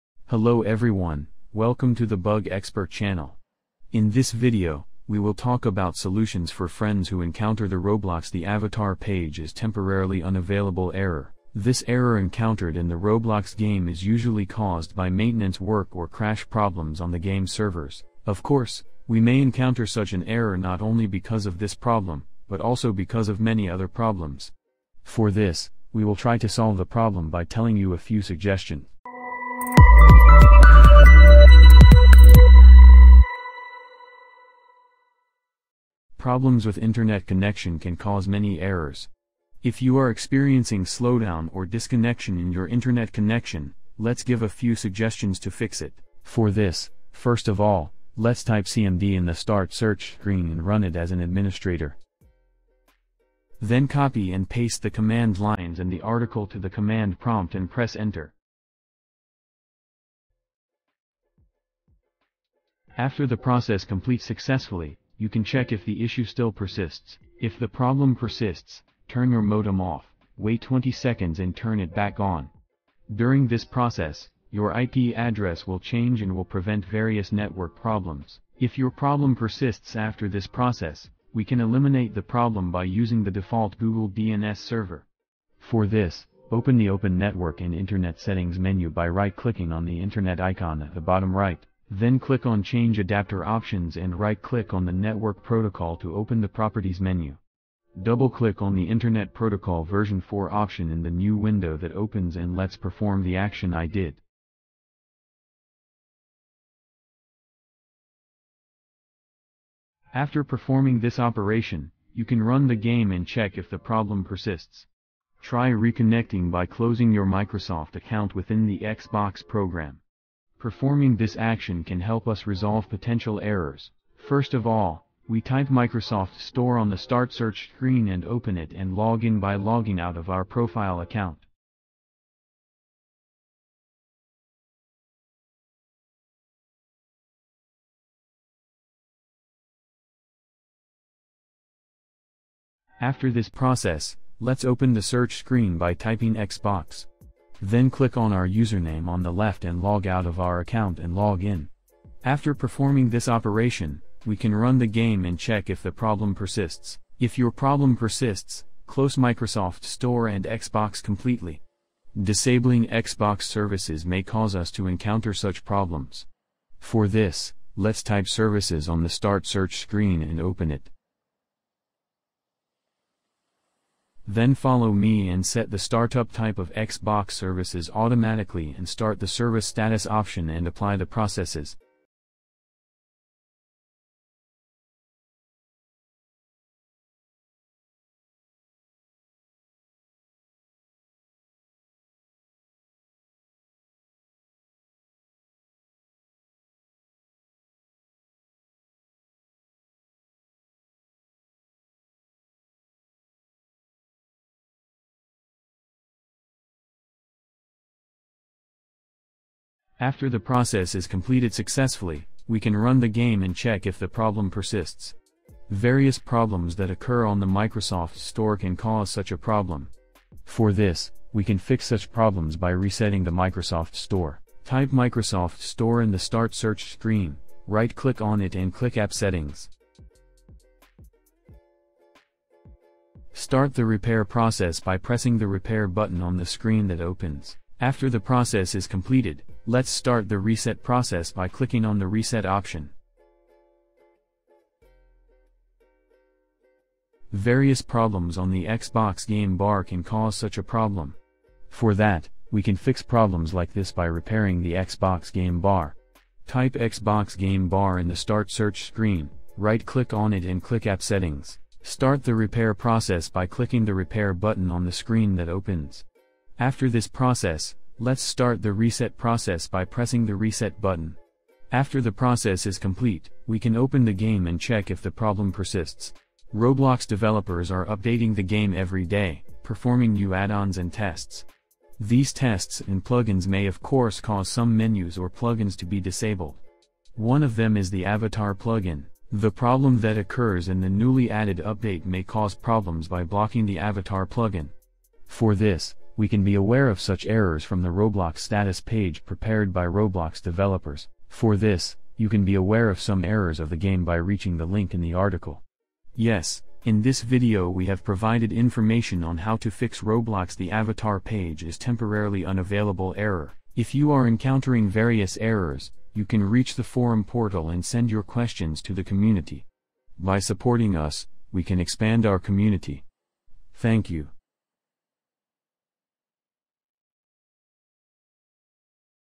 FIX: ROBLOX THE AVATAR PAGE IS TEMPORARILY UNAVAILABLE NEW
In this article, we will try to resolve the "Avatar page is temporarily unavailable" error that Roblox players encounter when trying to access the avatar page.